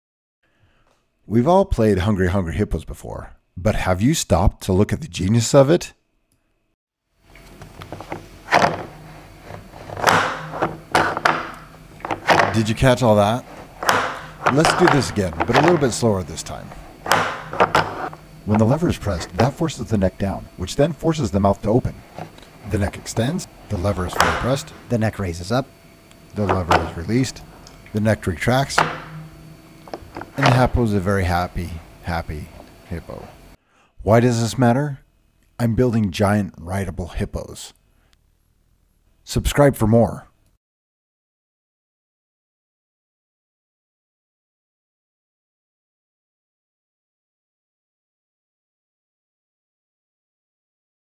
The Genius of Hungry Hungry Hippos
I'm working on building a massive 3d printed rideable version of Hungry Hungry Hippos game!

DrZzs may be compensated if using some of the links provided.